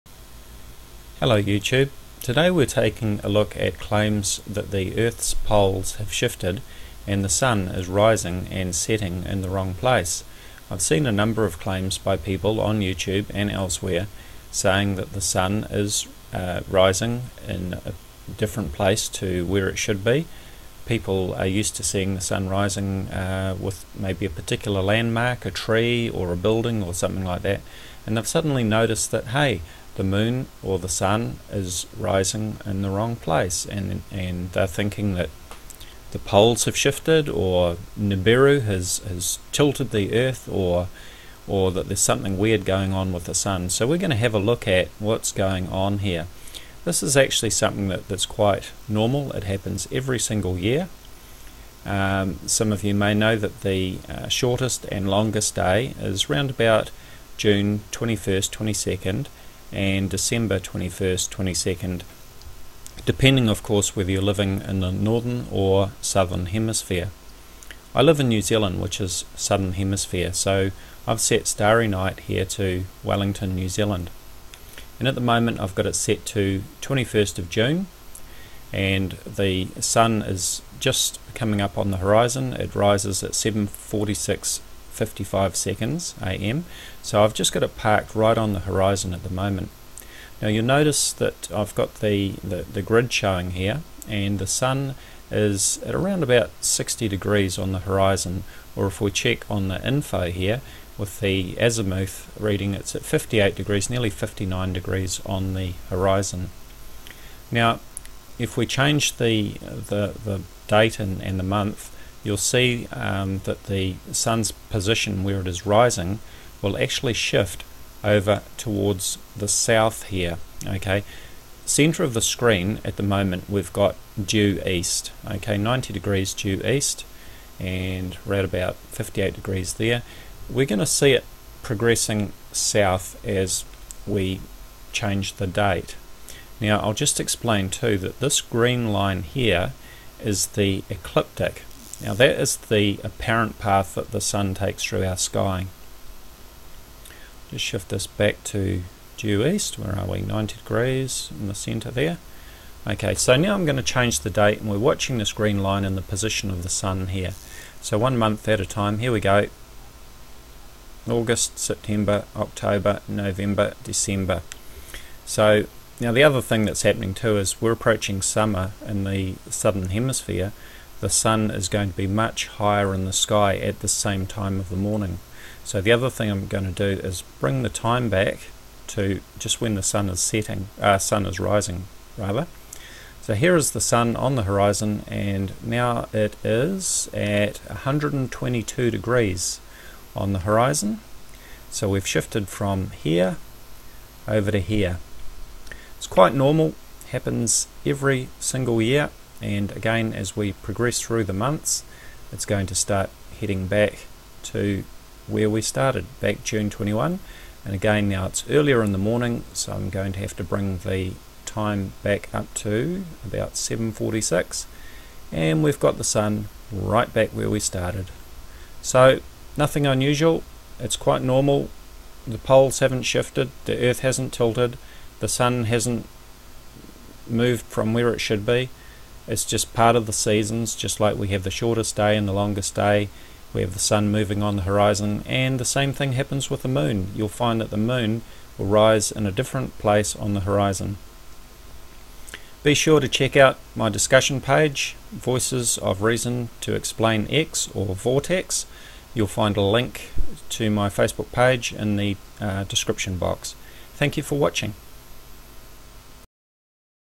Has Pole Shift Caused the Sun to Rise in the Wrong Place?
Is the Sun is rising and setting in the wrong place?  Has pole shift tilted the Earth and caused the sun to rise in the wrong place?  Has the moon's orbit been affected by Nibiru?  We take a look at what is going on using StarryNight Pro.